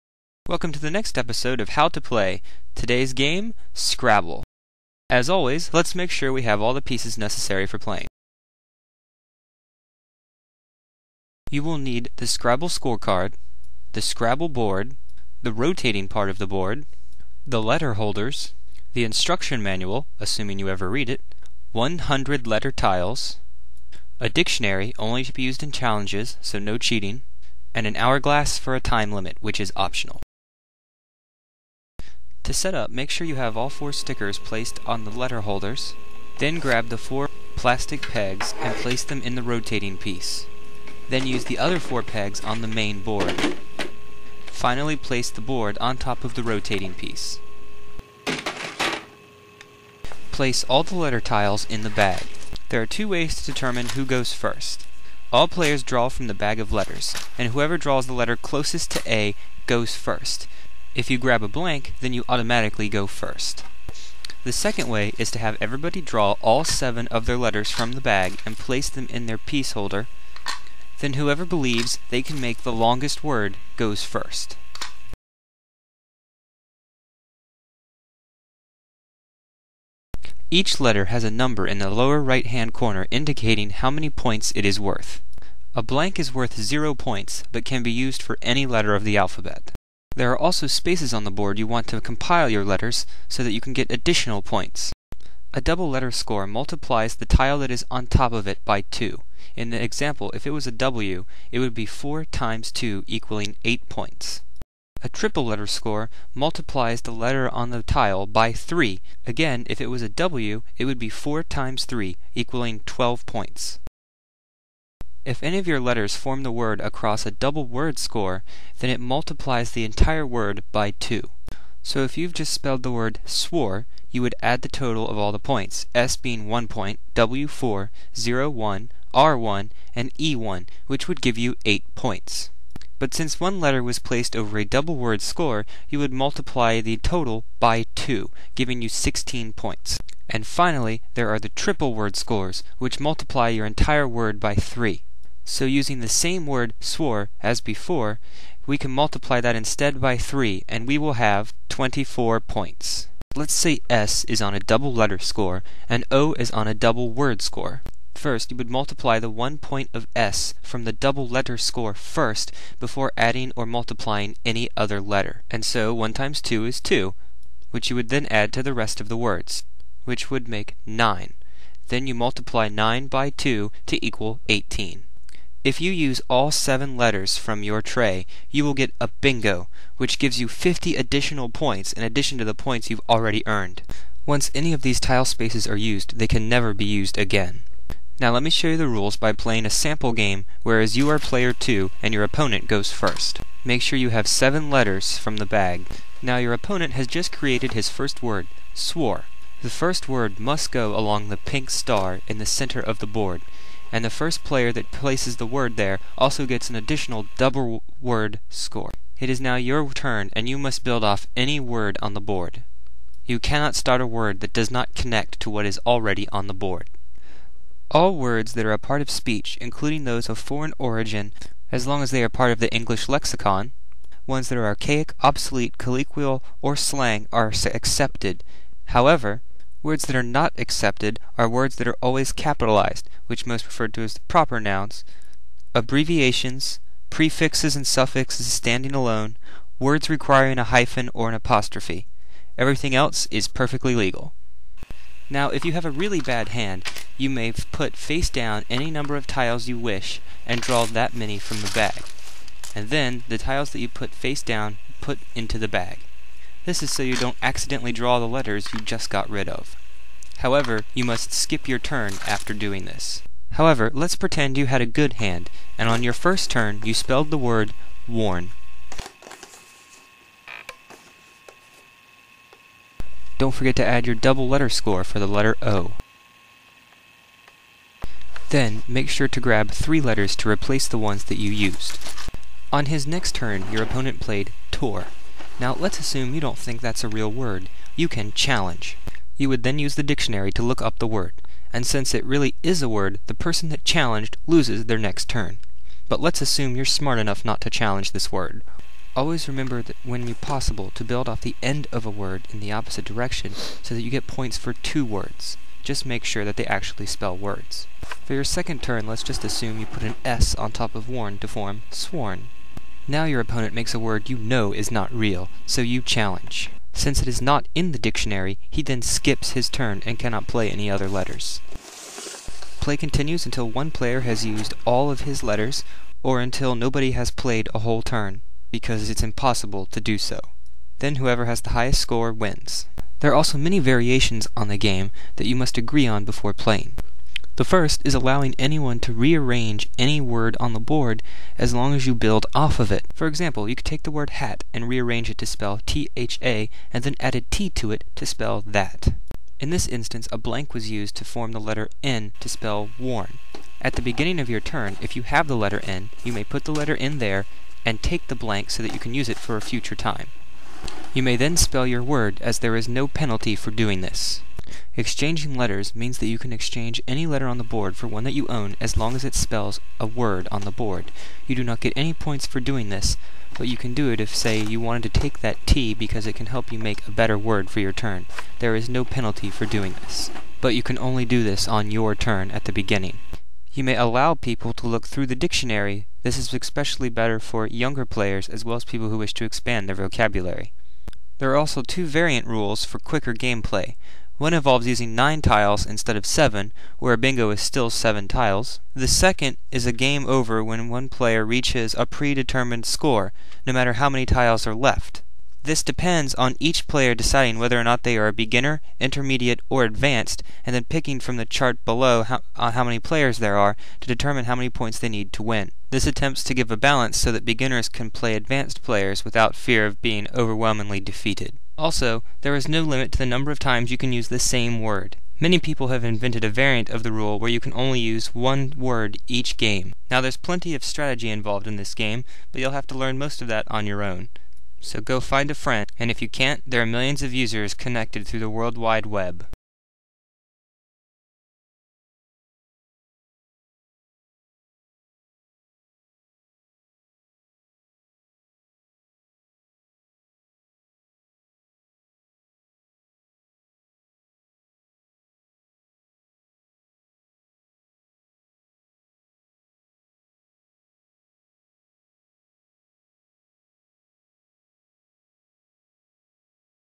How To Play - Episode 4 - Scrabble
Yes, this is how to play Scrabble.  Stick around and find out some variations on the rules you never knew existed (maybe you didn't read the instruction manual).